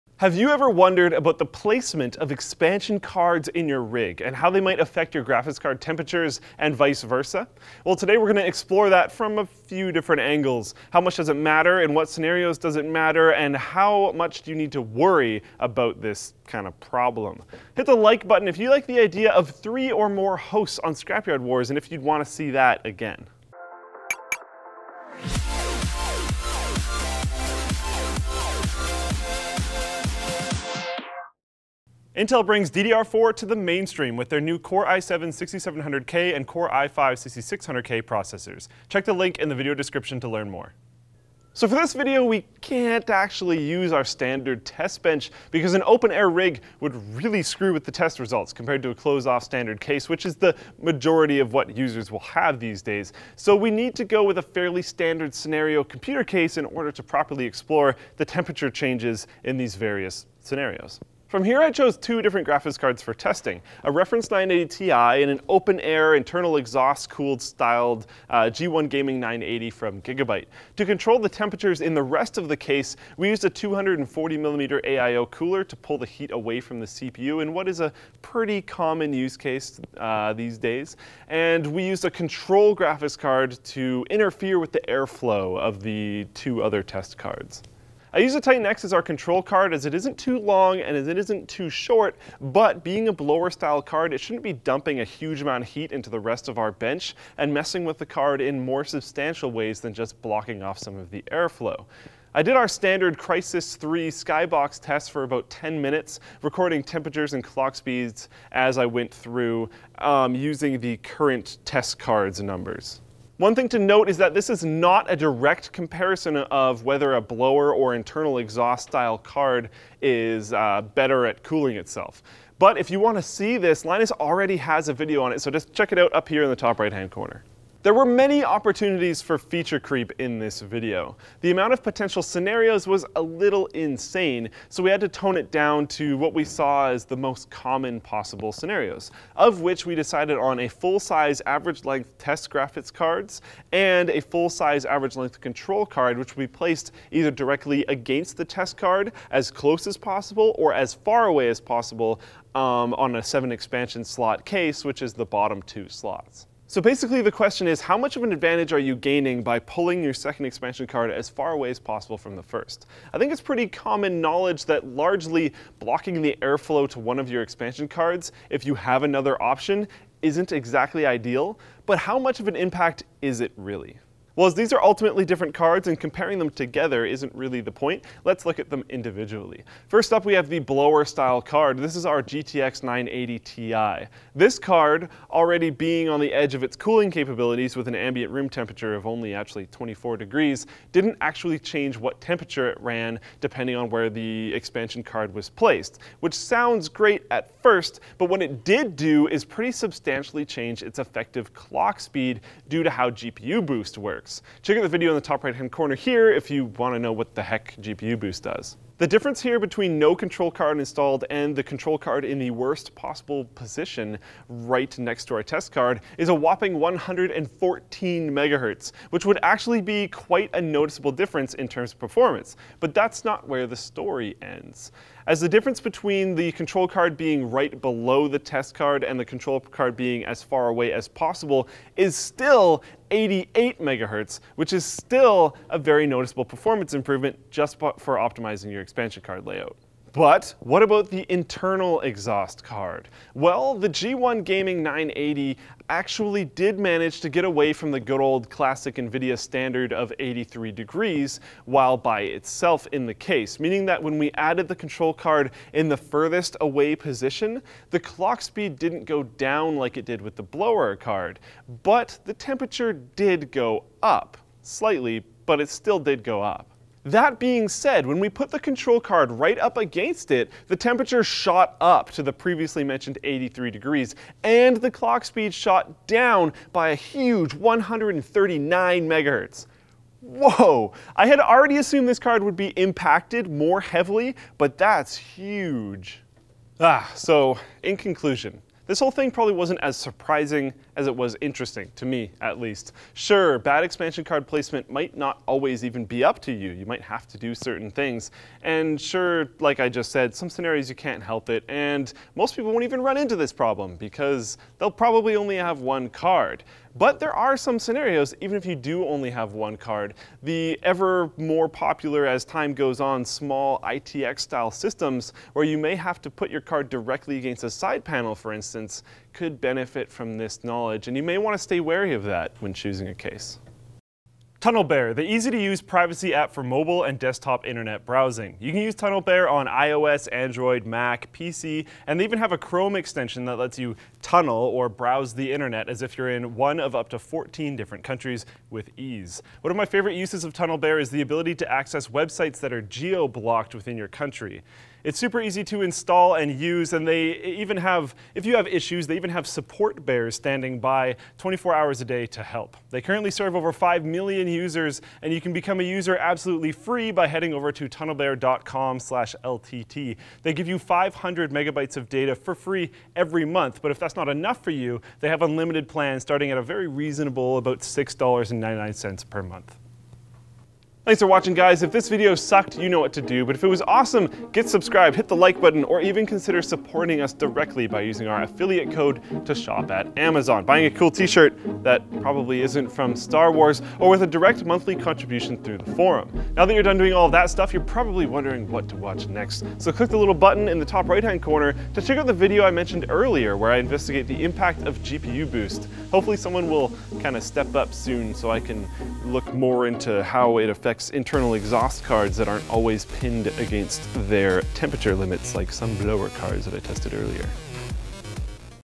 Video Card Ventilation - Does card proximity affect temperatures?
Do other PCIe cards affect the temps of your video card? Should you be concerned with "feng shui-ing" your build for optimal airflow?